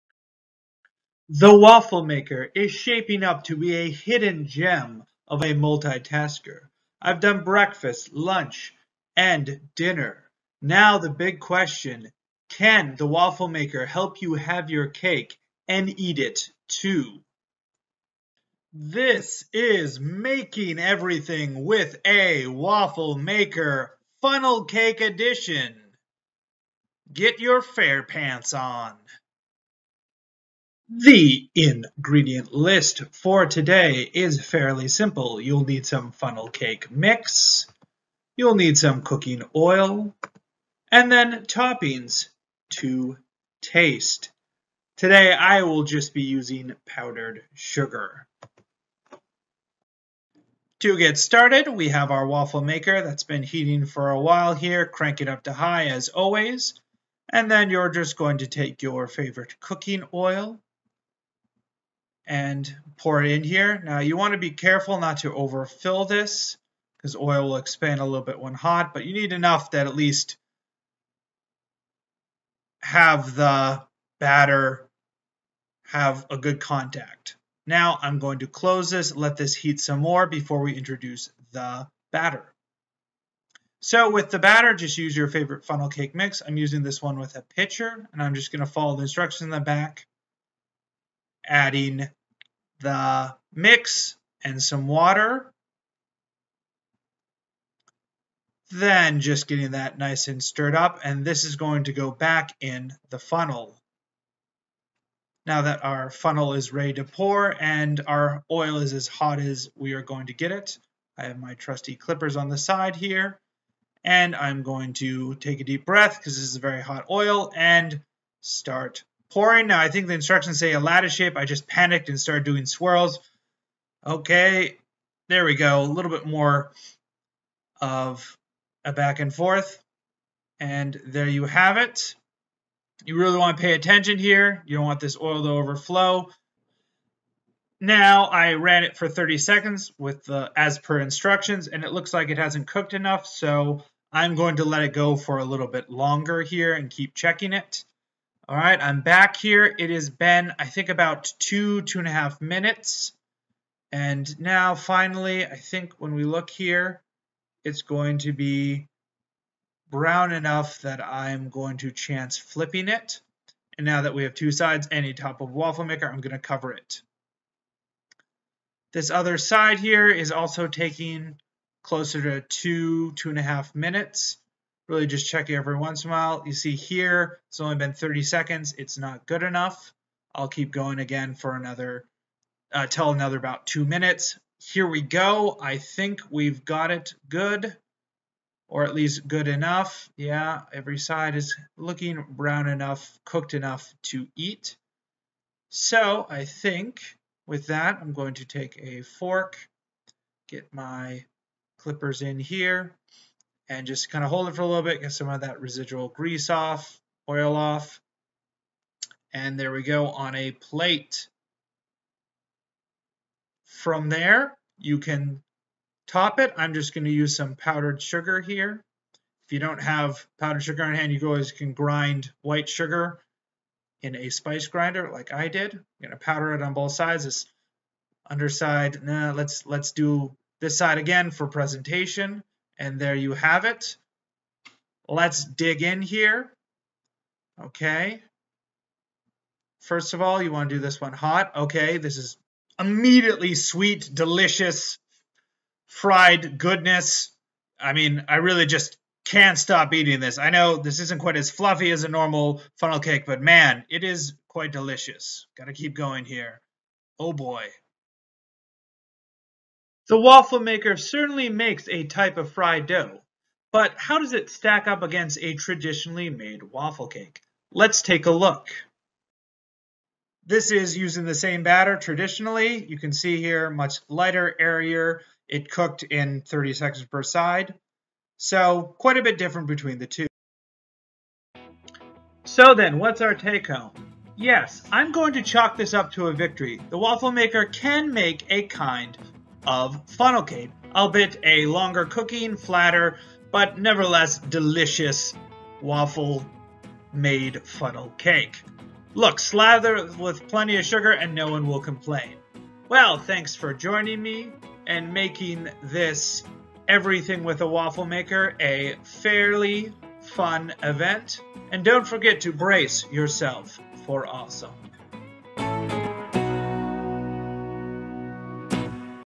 Making Everything with a Waffle Maker: Funnel Cake
Can you make everything with a waffle maker?  Get your tickets, step right up, and make sure to wait to ride the roller coaster until your stomach has settled.  It's funnel cake time!